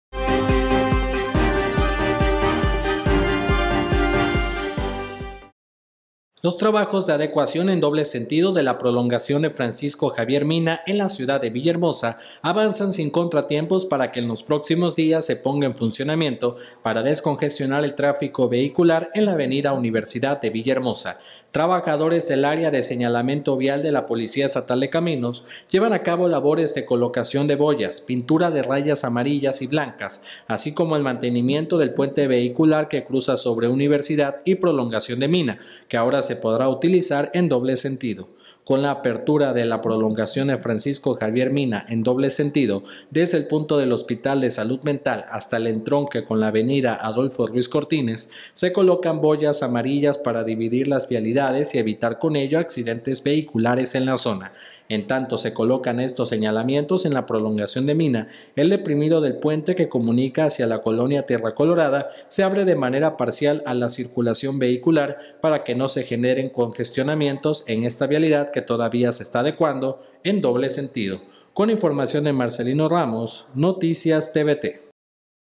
Continúa adecuación en doble sentido de la prolongación de "Francisco Javier Mina"
NoticiasTVT🟣 Avanzan son contratiempos los trabajos en la prolongación de 'Mina' para habilitarla como vía de doble sentido desde el bulevar 'Ruiz Cortines' hasta el puente que la une con avenida 'Ramón Mendoza' en Tierra Colorada.